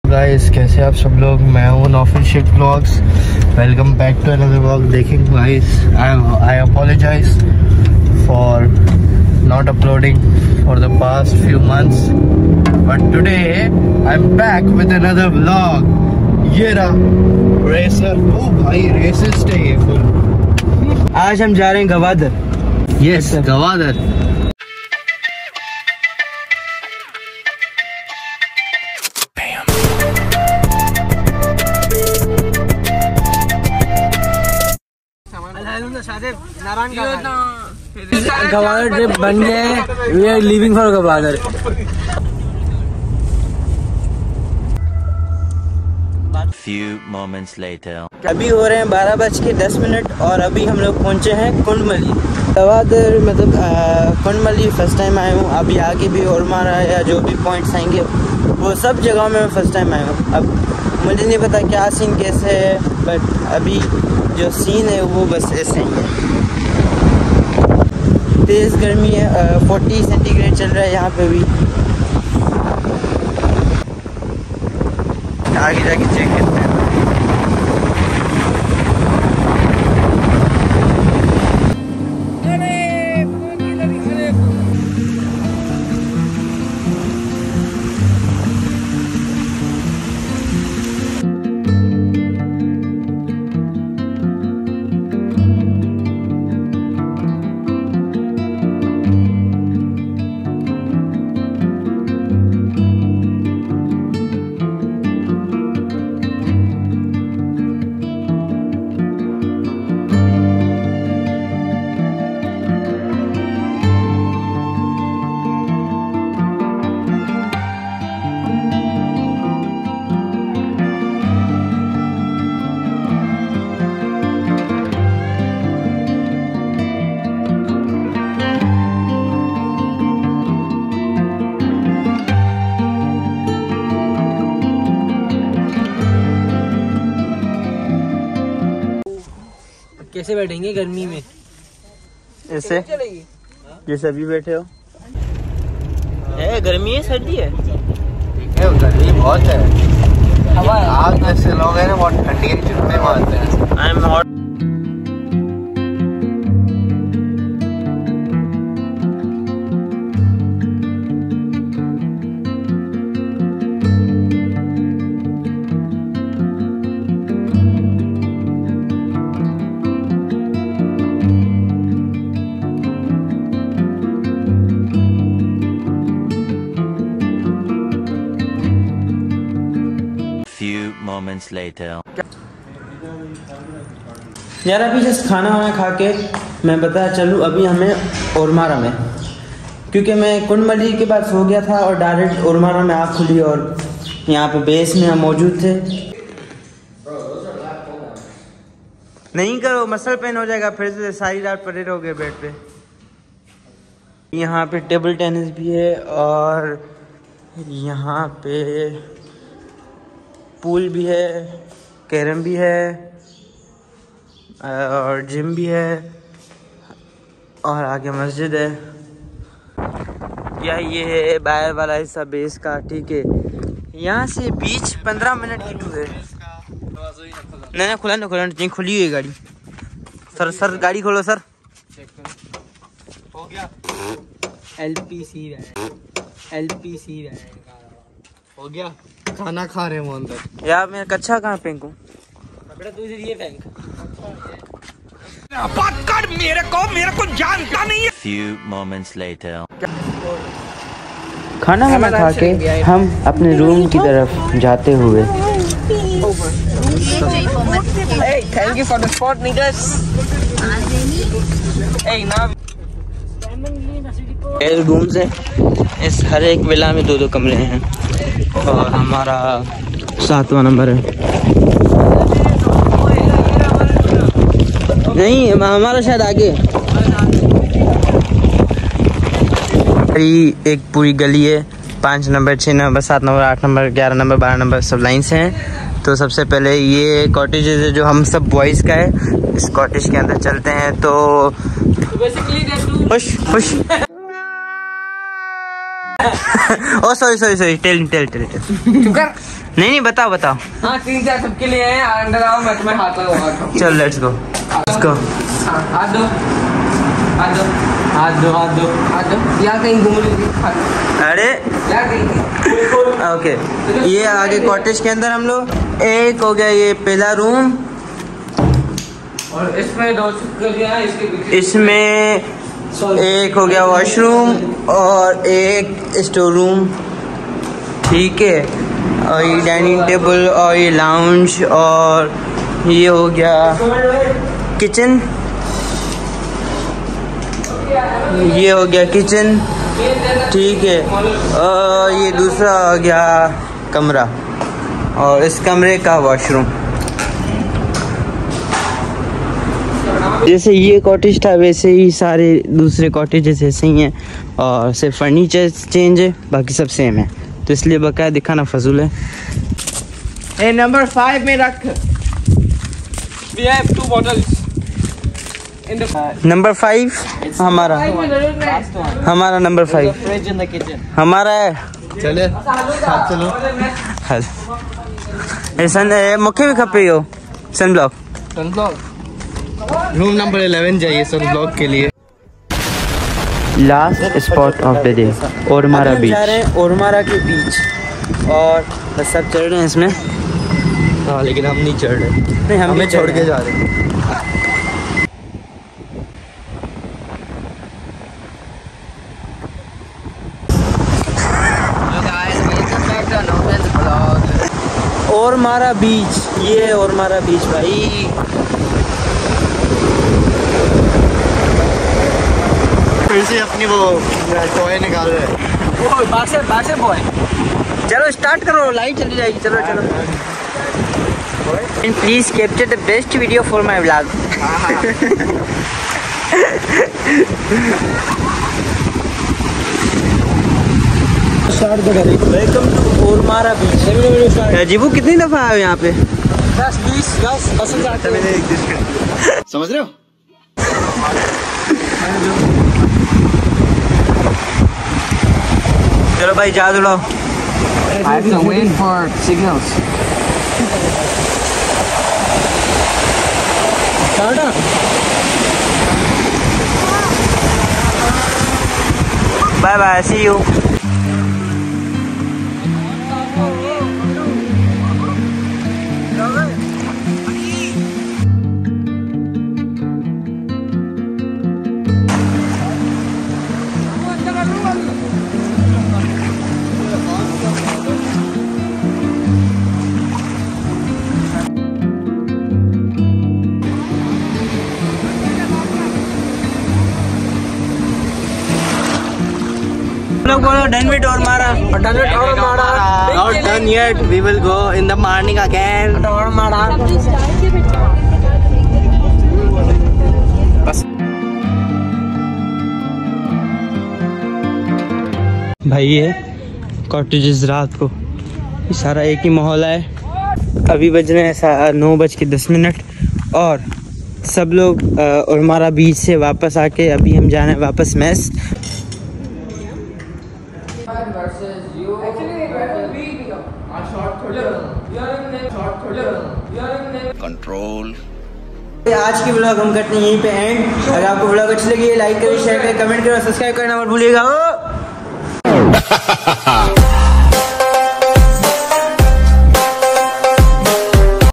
Kund malir and ormara / vlog / noufil shaikh
In this vlog we went to ormara and kind malir  then gwader